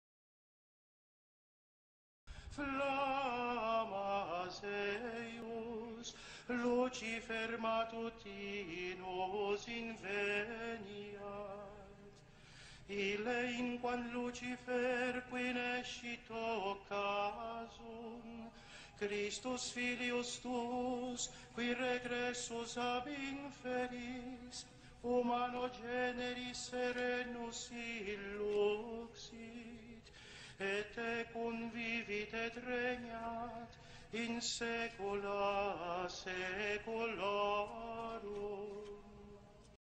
Der Vatikan und Luzifer
Ist Luzifer der Sohn Gottes? Ja, der Vatikan huldigt Yeshua beziehungsweise Luzifer